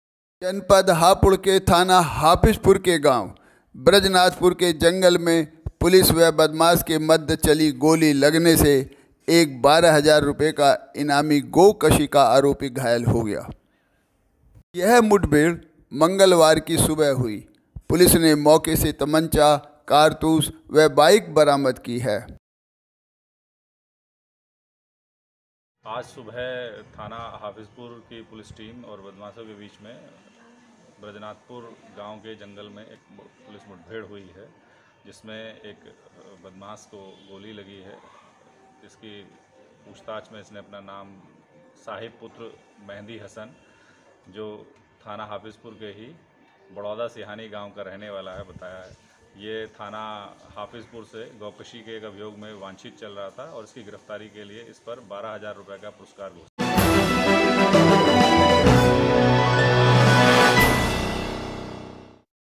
हापुड़:पुलिस मुठभेड़ में 12हजार का इनामी घायल || ehapur news
हापुड़:पुलिस मुठभेड़ में 12हजार का इनामी घायल.